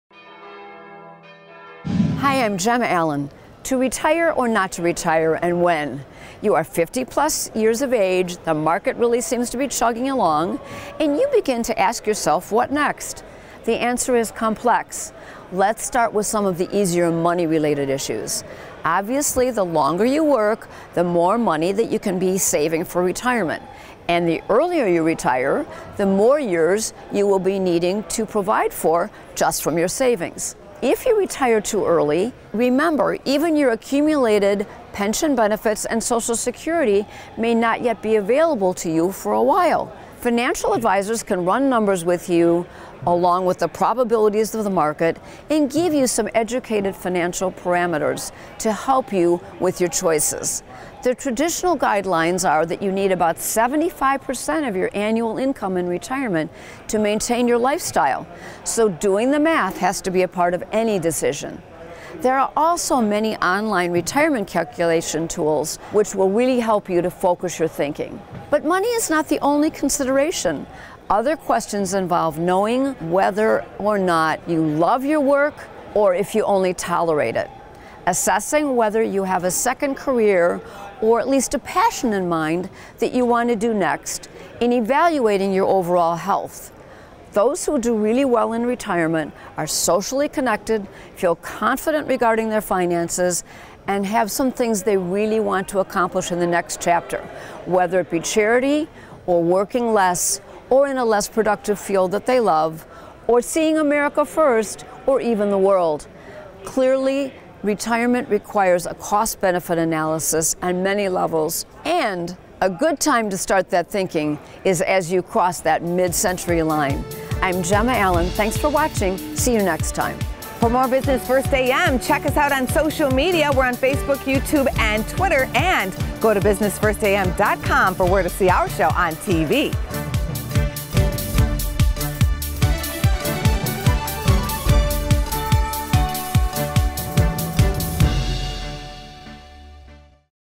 TO RETIRE OR NOT TO RETIRE?
Attorney Gemma Allen from Allen & Glassman gives you some great advice on when to retire and what you'll need before you make the leap!